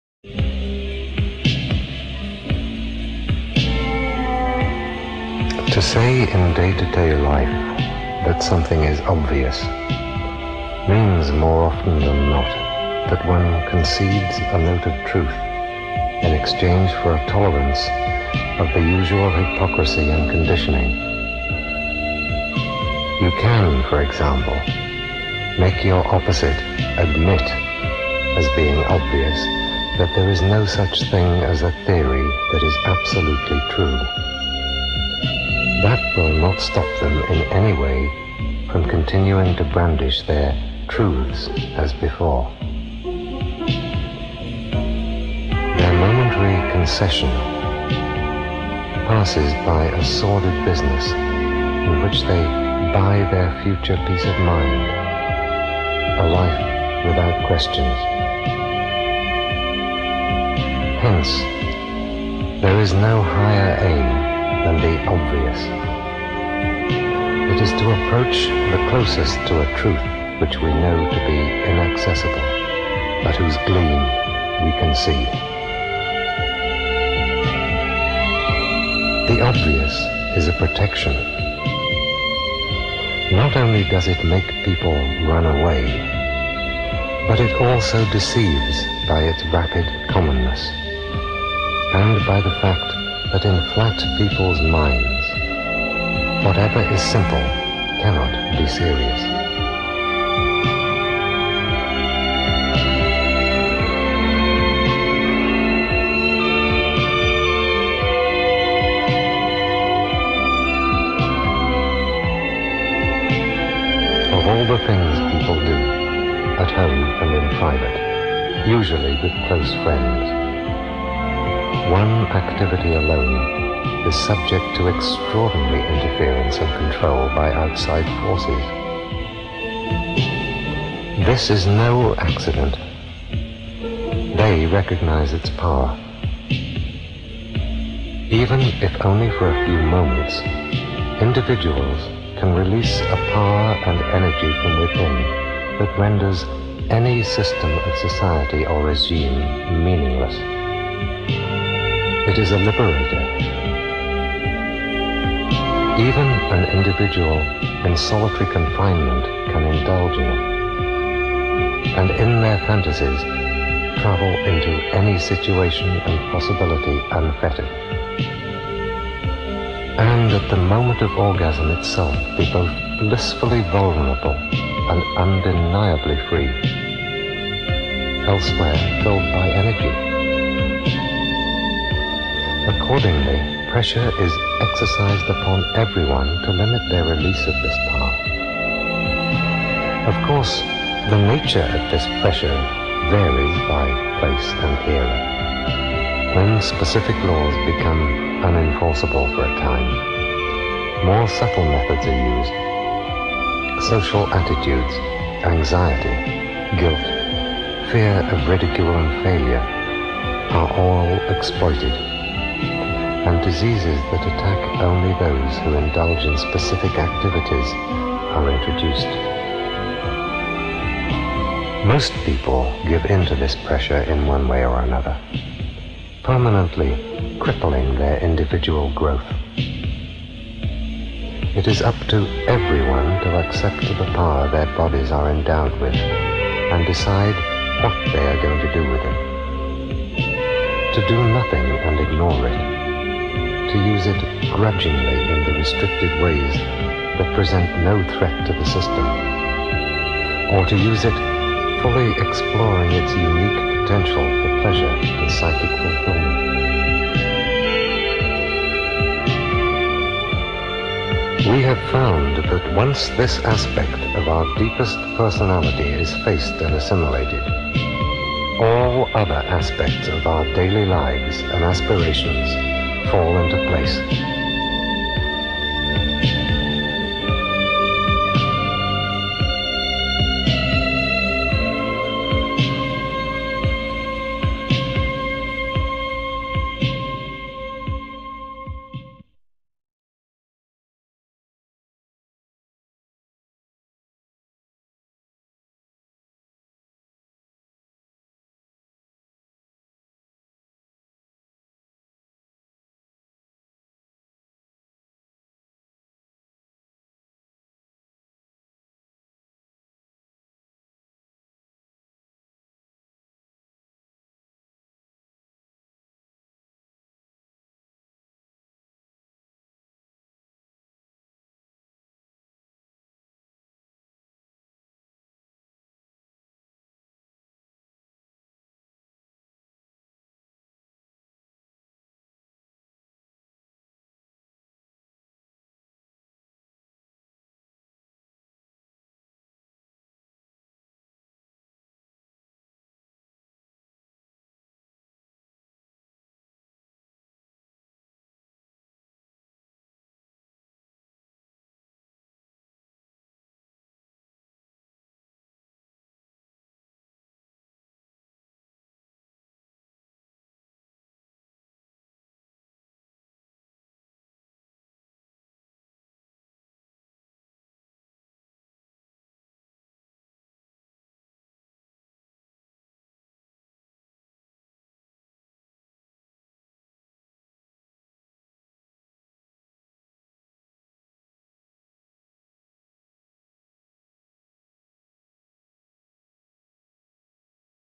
A Message from Thee Temple ov Psychic Youth, 1983
A message from the now defunct Temple ov Psychic Youth, founded by Genesis Breyer P-Orridge.